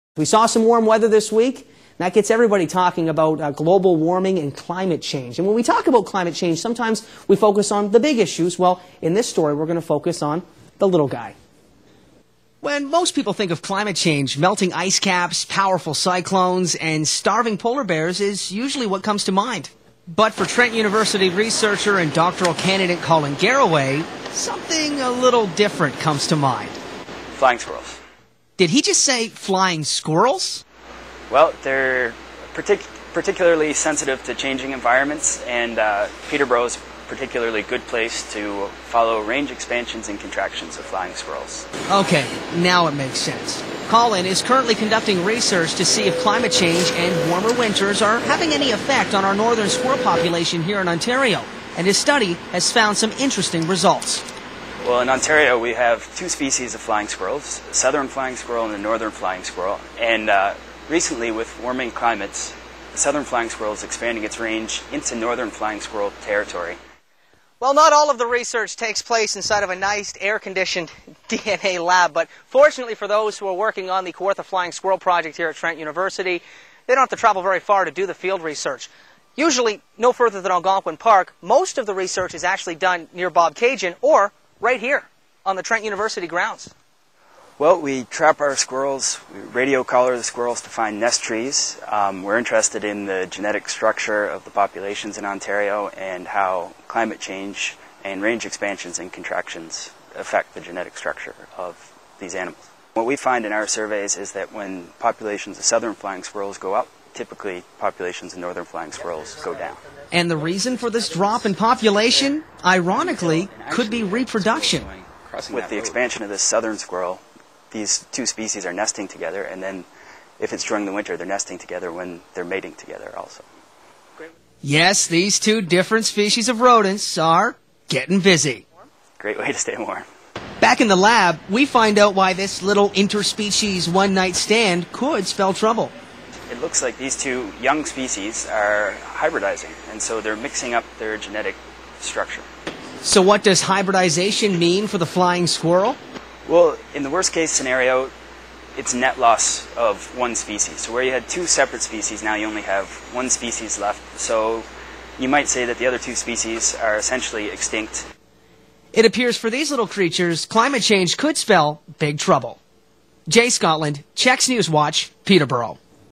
"Hybrid" Squirrels
In this feature we talk to one Trent University researcher about his work relating to climate change and Ontario's flying squirrel population.  Sound nerdy enough for you?  Well it's actually really interesting and Colin's work is getting a lot of attention.  Very cool!